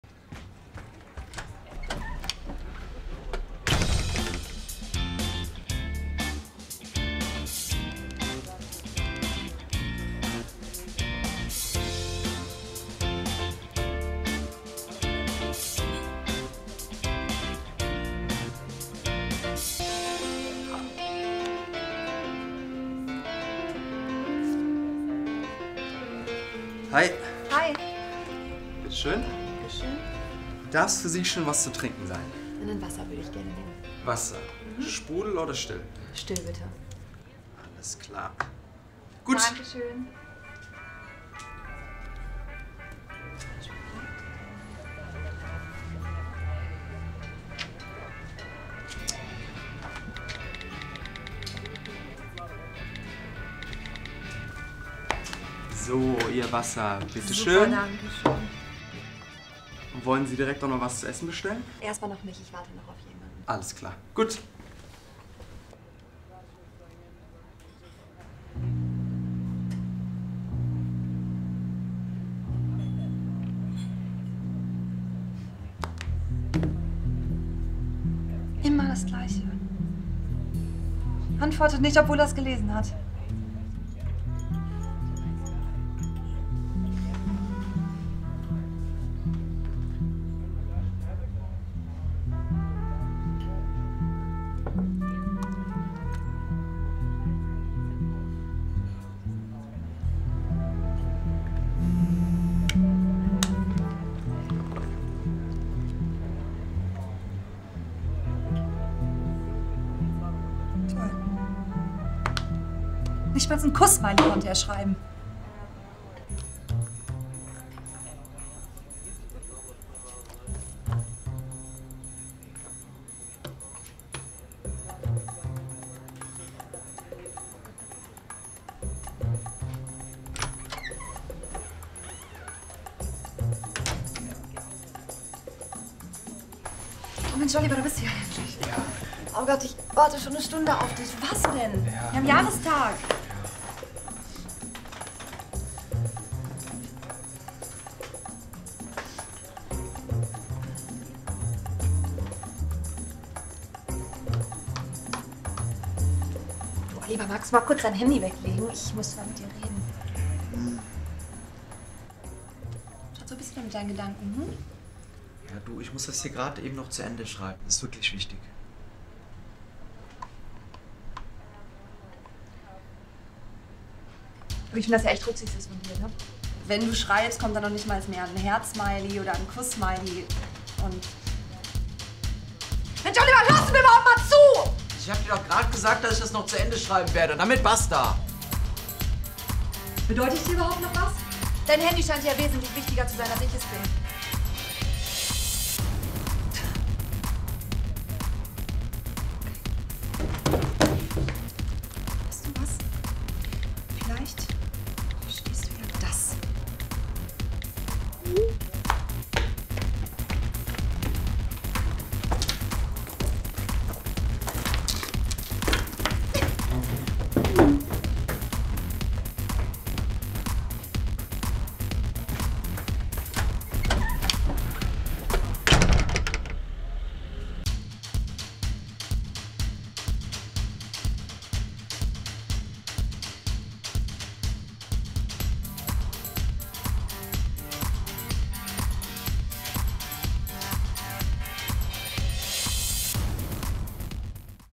Lautlos - Kurzfilm
Der Kurzfilm ist im Rahmen meiner Ausbildung zur Mediengestalterin Bild und Ton zum Thema "LOL&Co - Verstehen wir uns eigentlich noch?" entstanden.

Danke an die Schauspieler Isabelle Haase, Michael Hintzen und Frederik Winterhager, sowie meine Crew Sandra Höfler und Florian Hackbarth und der Sakleti Bar als Drehort! :)